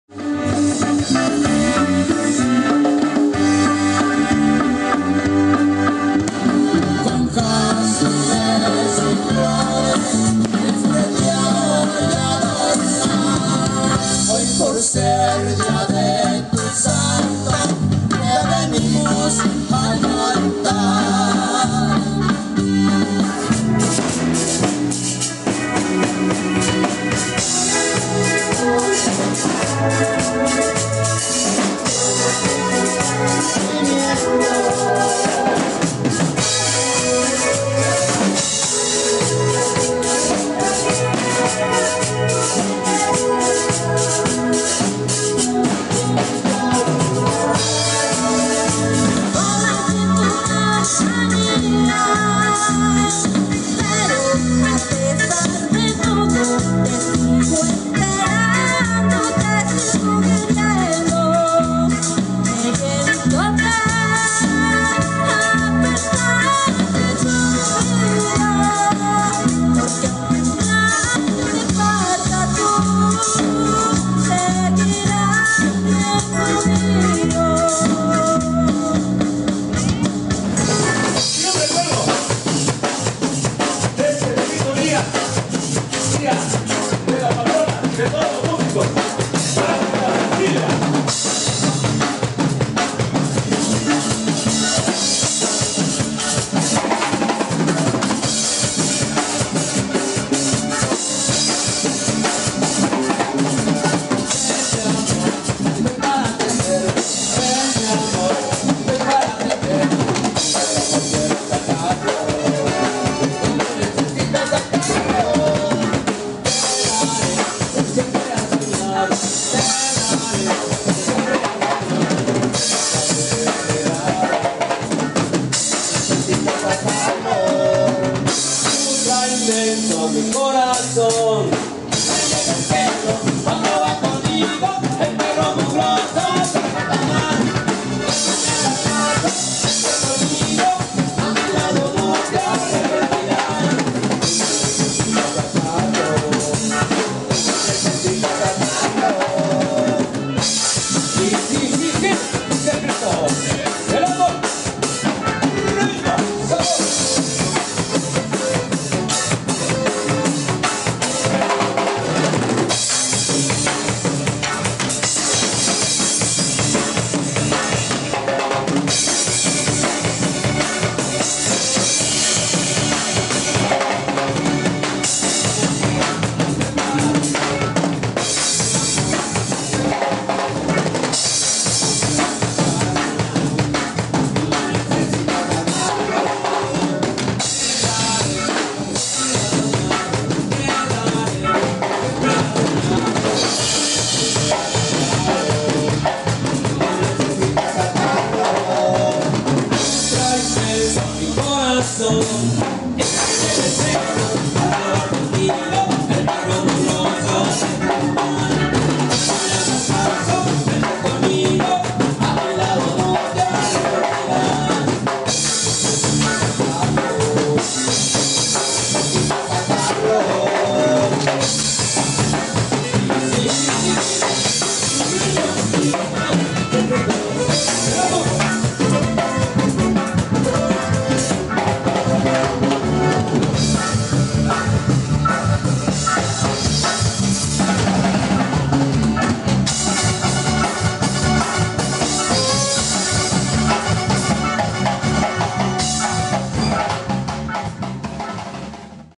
Secretos del Amor / Tanhuato 2017
Presentación del grupo "Secretos del Amor" durante el festejo en honor a Santa Cecilia el 22 de noviembre en la plaza principal de Tanhuato.